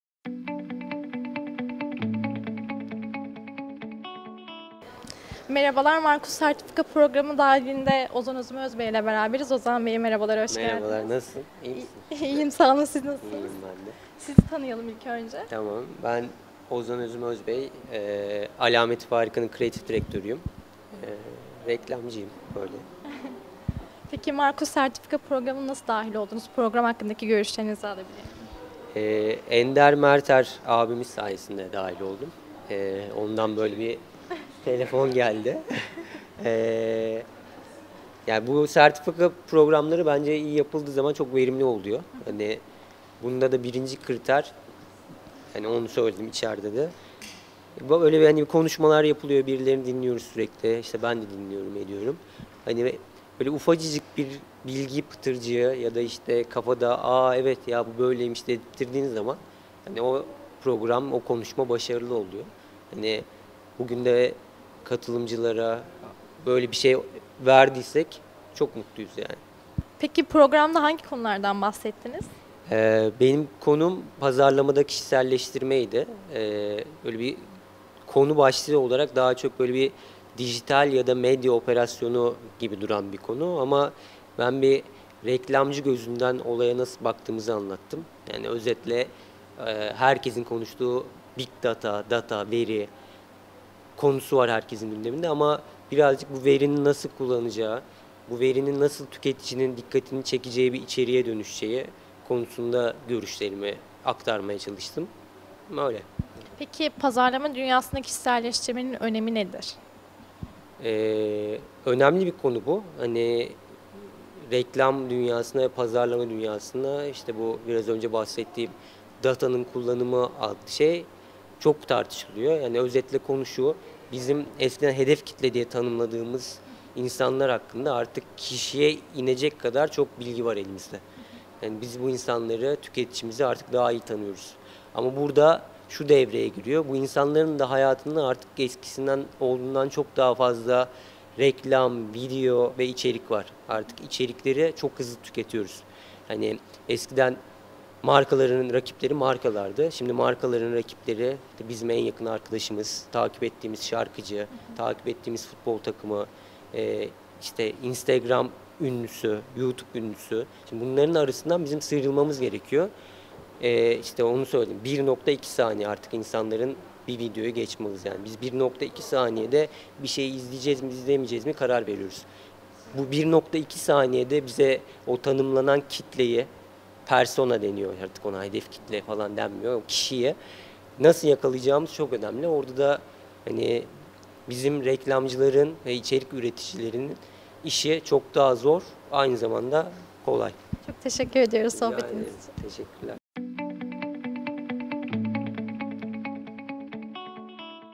MARKUS-OZAN ÖZÜM ÖZBEY
Marka ve reklam ustalarının, İstanbul Ticaret Üniversitesi Hayat Boyu Öğrenme Merkezi ve İlancılık Reklam Ajansı hayata geçirdiği sertifika programı.

Türkiye’nin önde gelen marka ve reklam ustalarının yer aldığı sertifika programı MARKUS, 9 şubatta başladı. 16 ve 23 Şubat tarihlerinde devam edecek olan ve Marketing Türkiye’nin desteğiyle gerçekleştirilen MARKUS sertifika programının moderatörlüğünü Ender Merter ile Doç.Dr. Gülay Öztürk yapıyor.